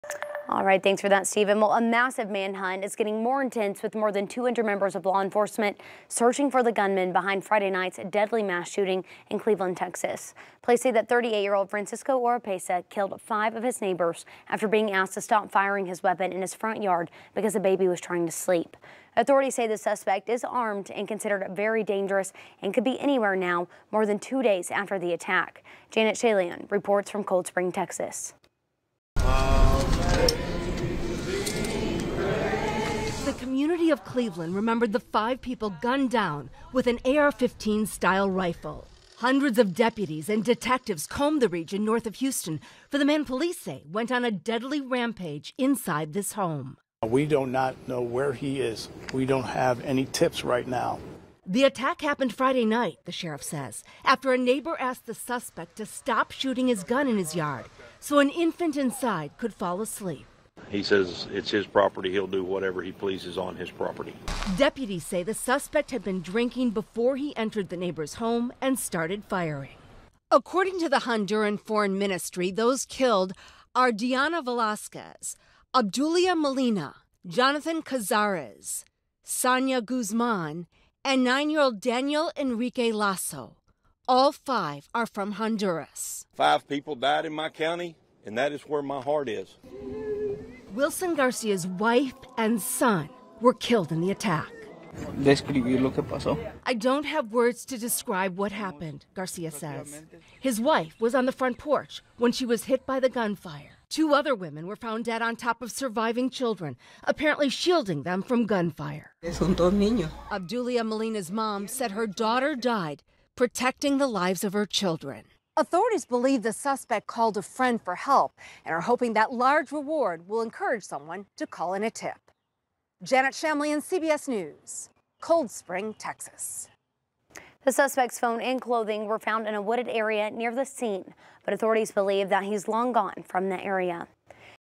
Search ongoing for man suspected in Texas shooting
Police in Texas continue to look for the suspect in a shooting that left five dead.